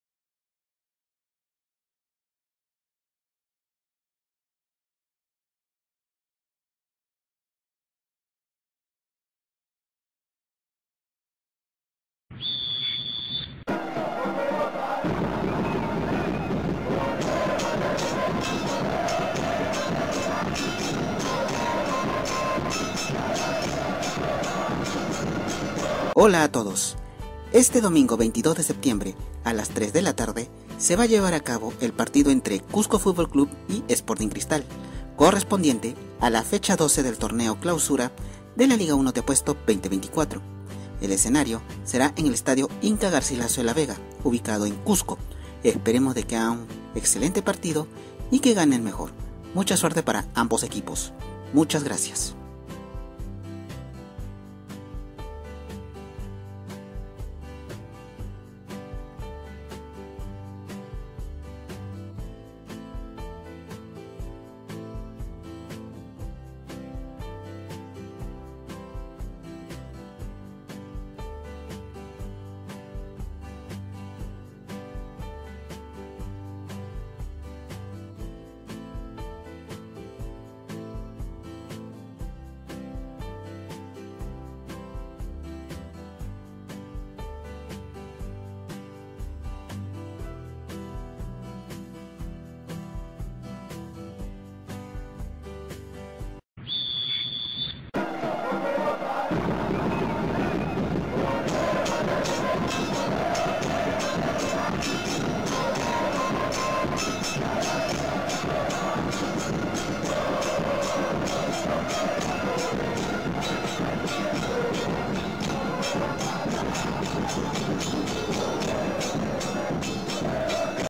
Torneo Clausura 2024 (Fecha 12): Cusco FC vs Sporting Cristal
Saludos amantes del deporte rey, esperemos de que todos ustedes estén alentando el partido entre "Cusco FC vs Sporting Cristal"; correspondiente a la fecha 12 del "Torneo Clausura de la Liga 1 Te Apuesto 2024". Mucha suerte para ambos equipos y que gane el mejor. Muchas gracias.